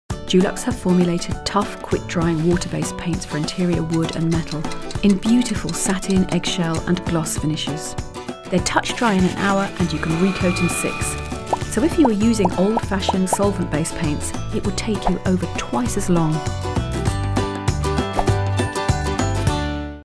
Dulux Quick Dry Satinwood
For long-lasting whiteness that’s touch dry in just one hour try Dulux water-based Quick Dry Satinwood for interior woodwork.